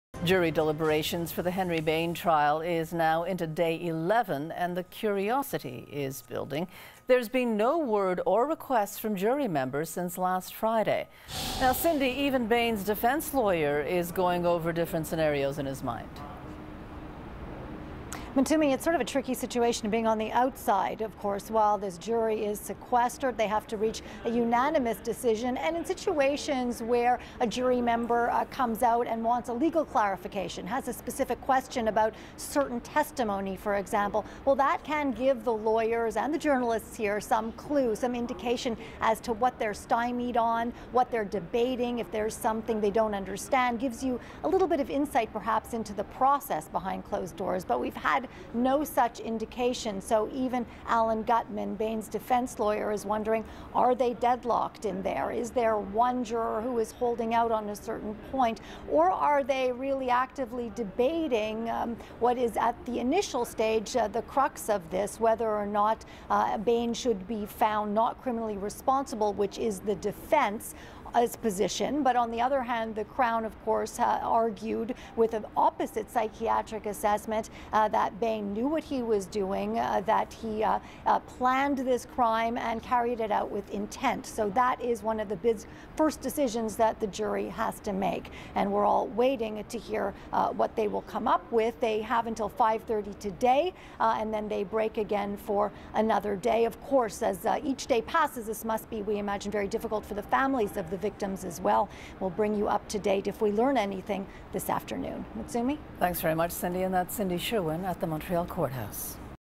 Shooting victim relieved Richard Bain found guilty
Shooting victim Dave Courage is relieved Richard Bain was found guilty.